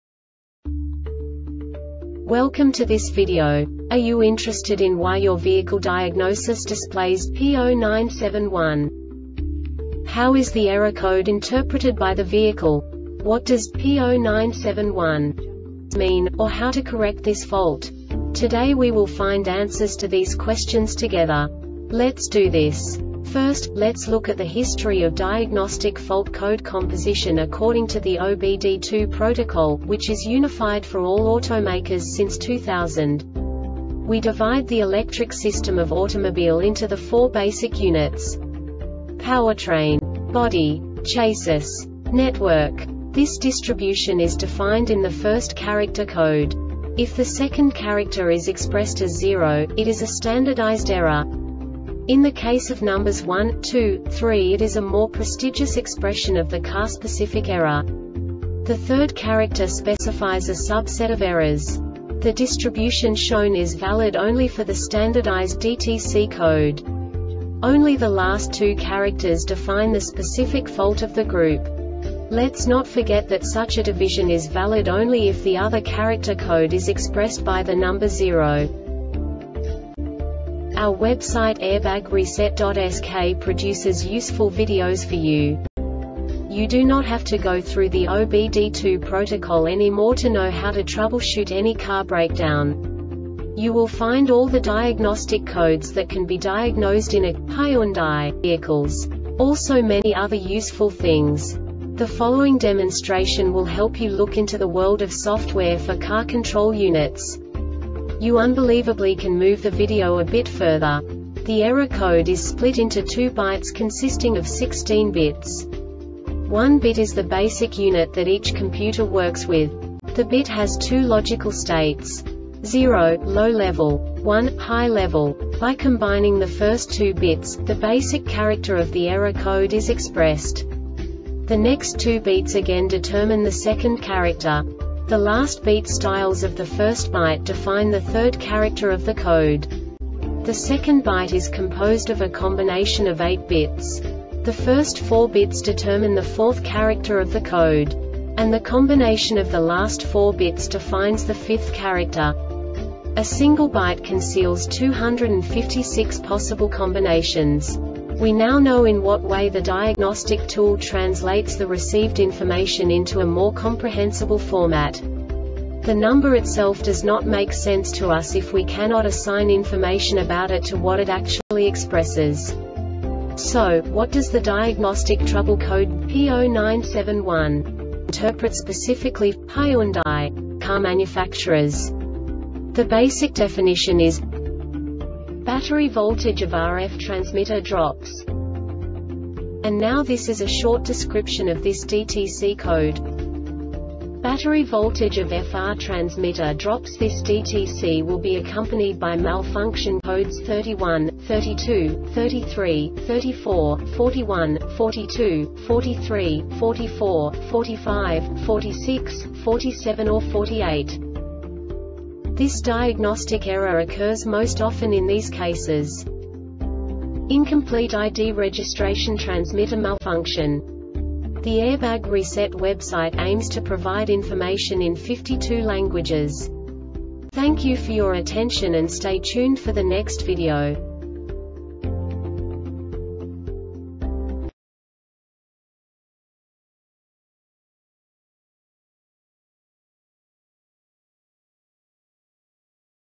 DTC Hyundai P0971 Short Explanation
Contents:
0:21 Basic DTC analysis according to OBD2 protocol standard.
1:48 Insight into programming
2:58 A diagnostically understandable description of the Diagnostic Trouble Code
3:05 Basic error definition

Make:
Hyundai
Code:
P0971
Definition:
Battery Voltage of RF Transmitter Drops.
Description:
Battery voltage of FR transmitter drops.This DTC will be accompanied by Malfunction Code(s): 31, 32, 33, 34, 41, 42, 43, 44, 45, 46, 47 or 48
Cause: